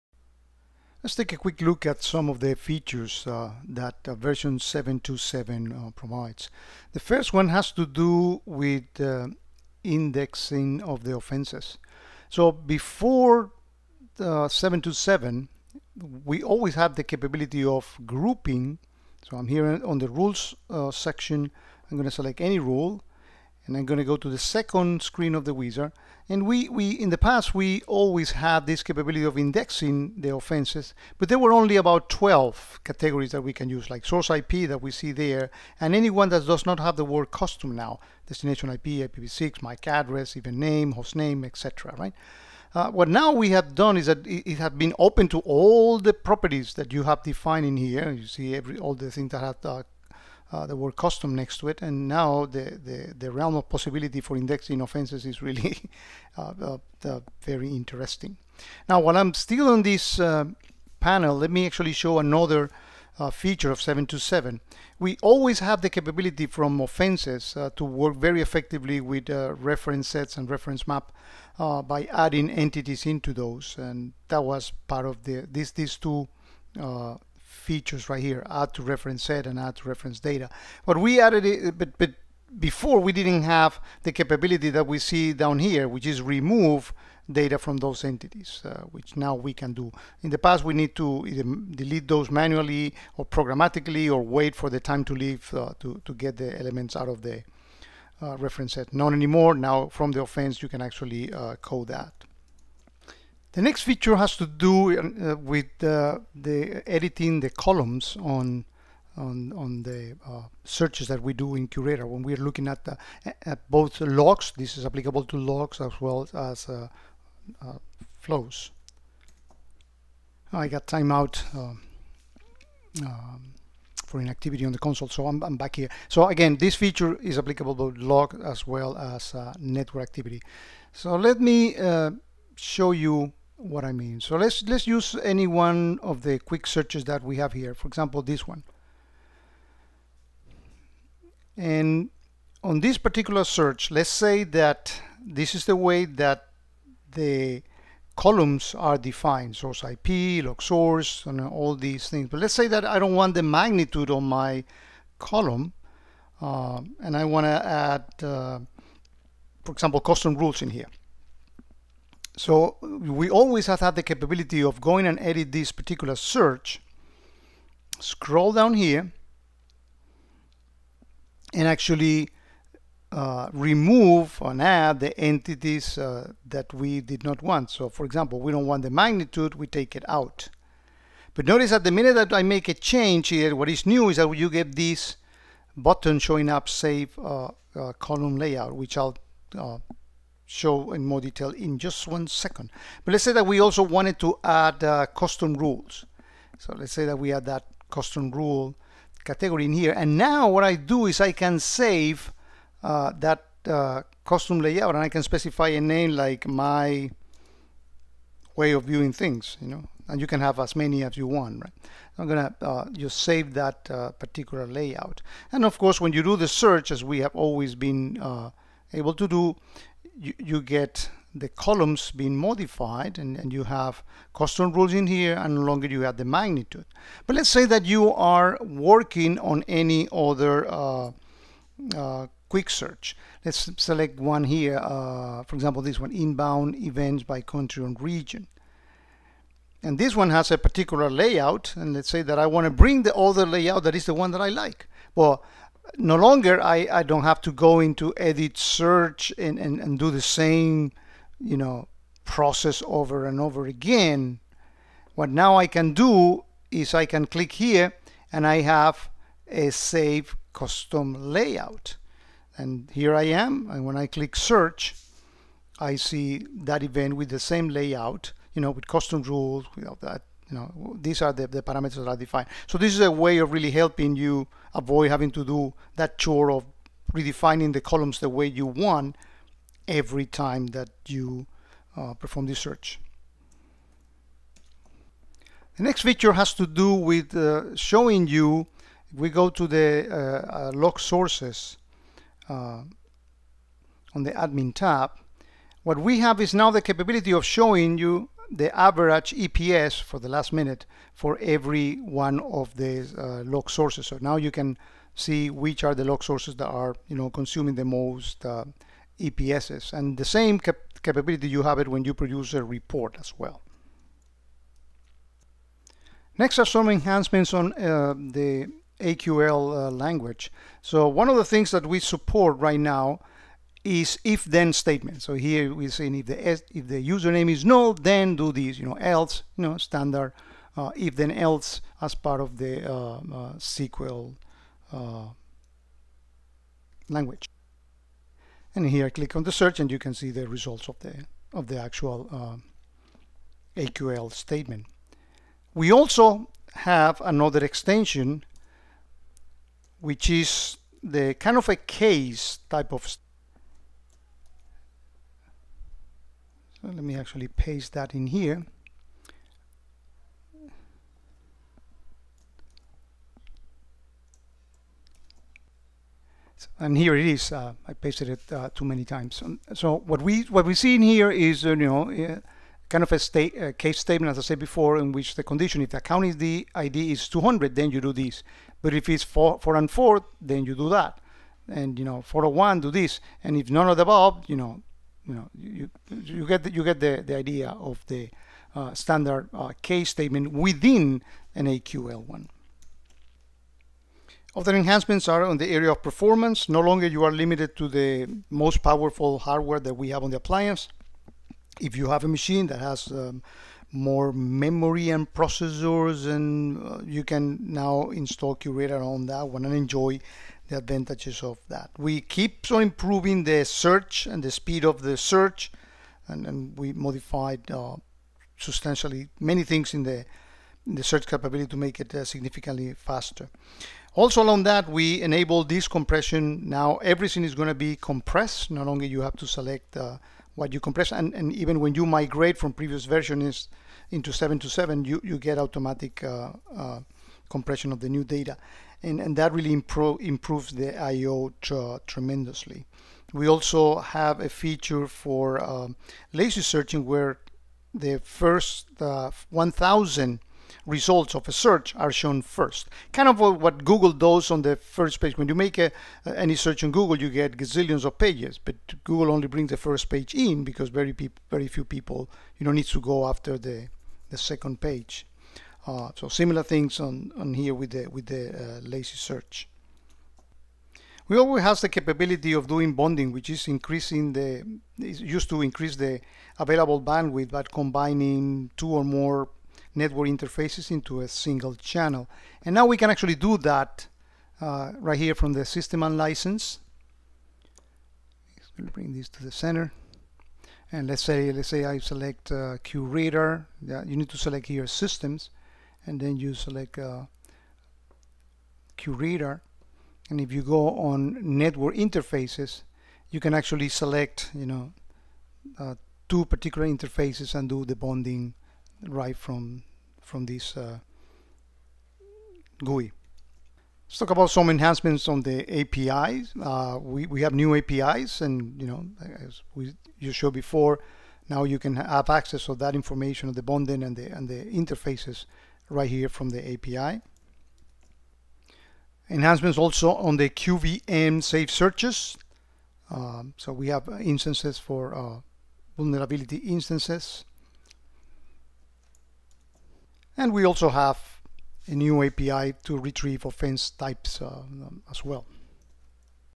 Qradar 7 2 7 Enhancements
Quick review of some of the most important enhancements for 7.2.7.
Notes: In the video I used the word Offenses and Rules as interchangeably which is not correct. Also, I would like to emphasize that because now all data is compressed, the amount of time that it takes to retrieve such data is much shorter, providing overall improvements on searches even comparing to prior 7.2.7 non-compressed data searches.